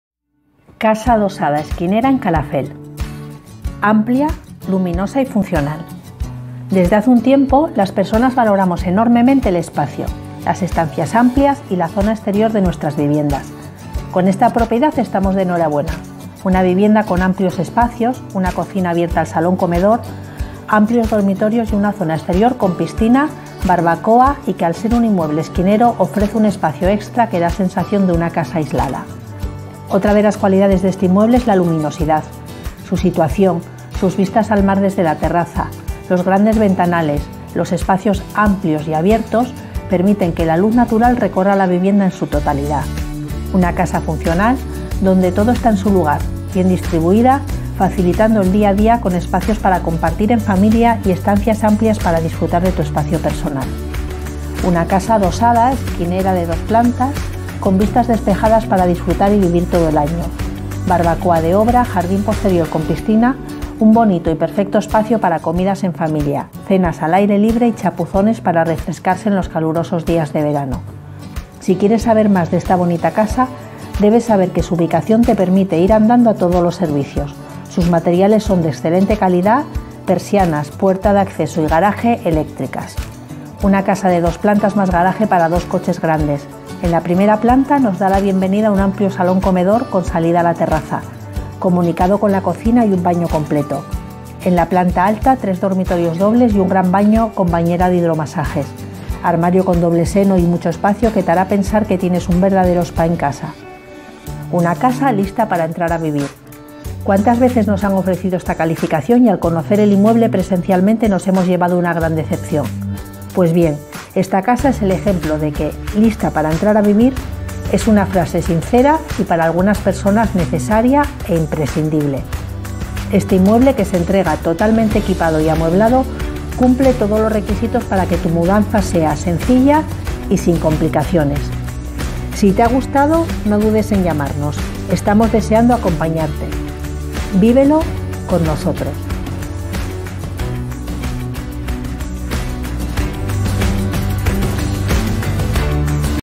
viveloconnosotros Casa Adosada Esquinera en Calafell
AMPLIA, LUMINOSA Y FUNCIONAL

Desde hace un tiempo las personas valoramos enormemente el espacio, las estancias amplias y la zona exterior de nuestras viviendas. Con esta propiedad estamos de enhorabuena, una vivienda con amplios espacios, una cocina abierta al salón-comedor, amplios dormitorios y una zona exterior con piscina, barbacoa y que al ser un inmueble esquinero ofrece un espacio extra que da sensación de una casa aislada.

Otra de las cualidades de este inmueble es la luminosidad, su situación, sus vistas al mar desde la terraza, los grandes ventanales y los espacios amplios y abiertos que permiten que la luz natural recorra la vivienda en su totalidad.

Una casa funcional donde todo está en su lugar, bien distribuida, facilitando el día a día, con espacios para compartir en familia y estancias amplias para disfrutar de tu espacio personal.

Una casa adosada esquinera de dos plantas, con vistas despejadas para disfrutar y vivir todo el año, comidas familiares, barbacoa de obra, jardín posterior con piscina.

Si quieres saber más de esta bonita casa debes saber que su ubicación te permite ir andando a todos los servicios, materiales de excelente calidad, persianas, puerta de acceso y garaje son eléctricas

Una casa de dos plantas mas garaje para dos coches grandes, en la primera planta nos da la bienvenida un amplio salón-comedor con salida a la terraza y comunicado con la cocina y un baño completo. En la planta alta tres dormitorios dobles y un gran baño con bañera de hidromasajes, doble seno y mucho espacio que te hará pensar que tienes un verdadero spa en casa.

ESPECIAL PORQUE...

Una casa lista para entrar a vivir, cuántas veces nos han ofrecido esta calificación y al conocer el inmueble presencialmente nos hemos llevado una gran decepción. Pues bien, esta casa es el ejemplo que "lista para entrar a vivir" es una frase sincera, y para algunas personas necesaria e imprescindible.

Este inmueble que se entrega totalmente equipado y amueblado cumple todos los requisitos para que tu mudanza sea sencilla y sin complicaciones.

Si quieres concertar visitas